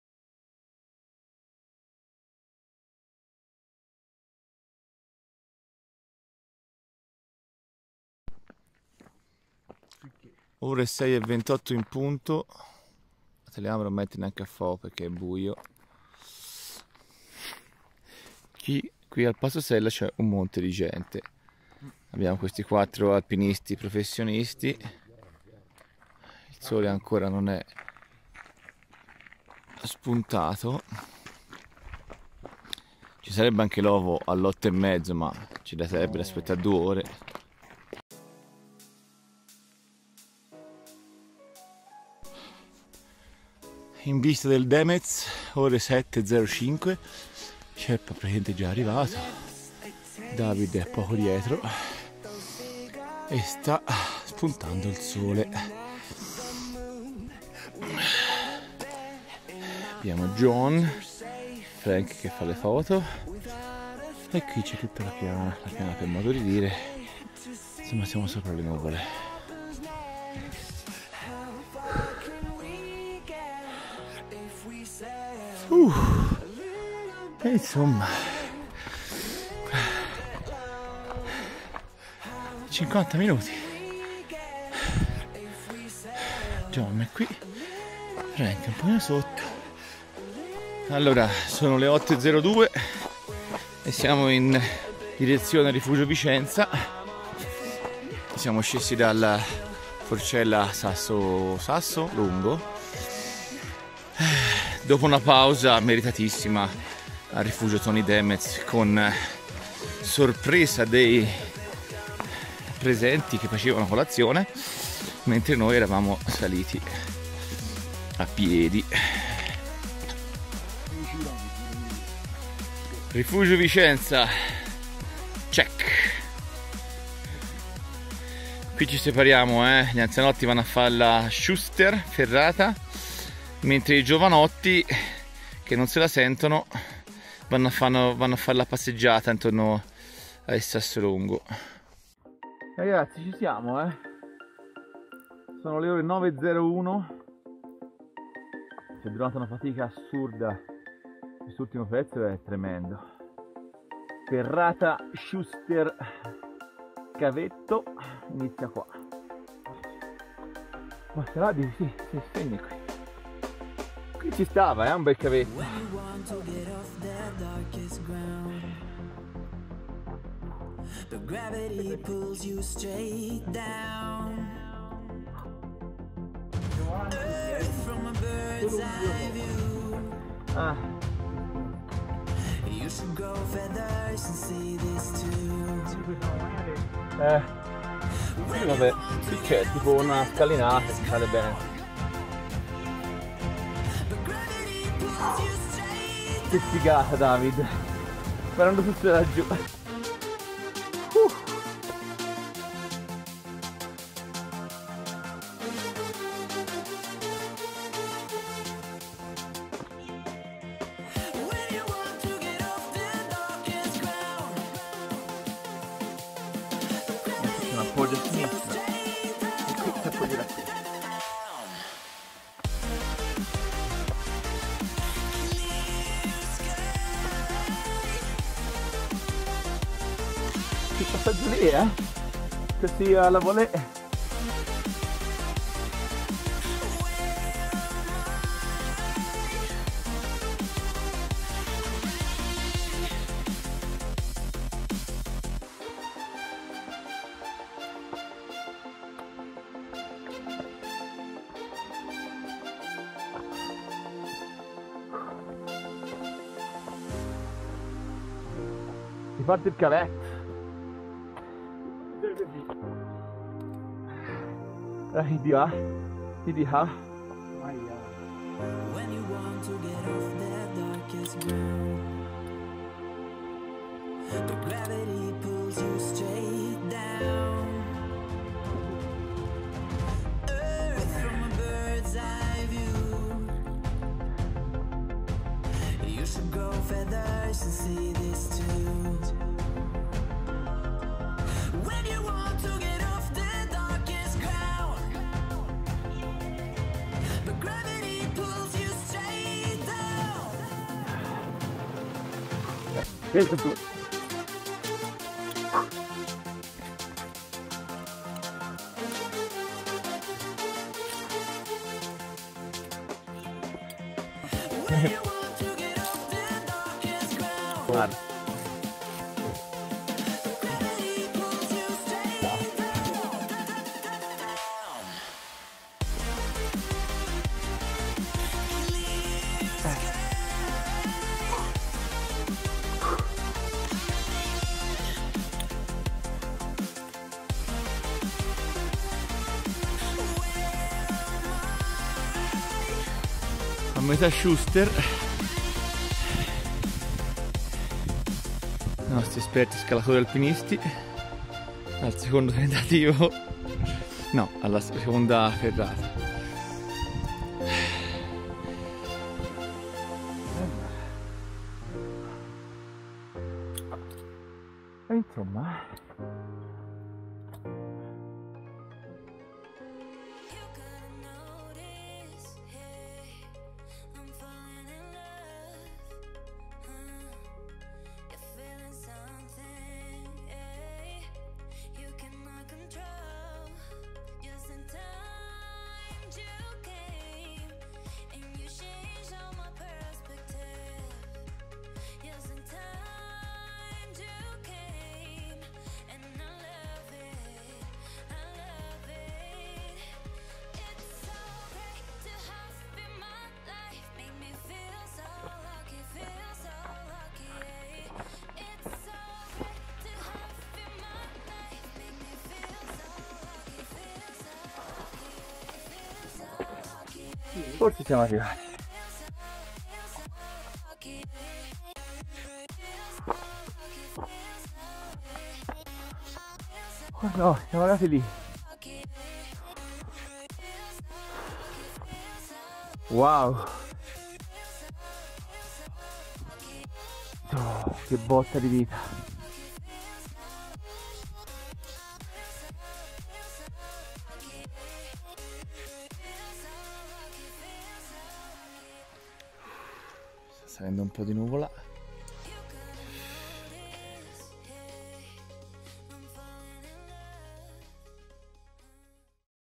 Trekking Dolomiti Ferrata Schuster 20.09.2020 Insta 360 One R
Per i video usiamo una GoPro 8 e una DJI Osmo 4k

00:00 Fontanazzo
00:10 Passo Sella
00:43 salita al Rifugio Toni Demetz
01:30 verso Rifugio Vicenza
02:23 Ferrata Schuster
07:10 Sasso piatto
60 One R